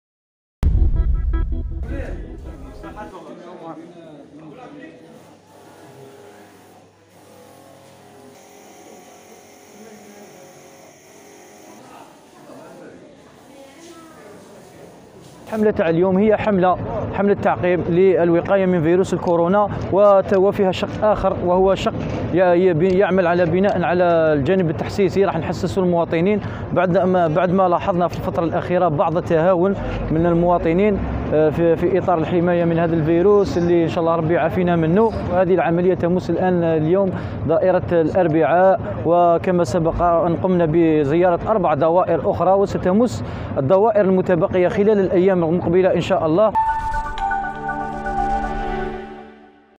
البليدة - كورونا: عملية تعقيم بالمكاتب الإدارية للأربعاء
البليدة (وأج)- عرفت مدينة الأربعاء (ولاية البليدة) عملية تعقيم مست المكاتب الإدارية، وهذا كإجراء وقائي من تفشي فيروس كورونا بين المواطنين.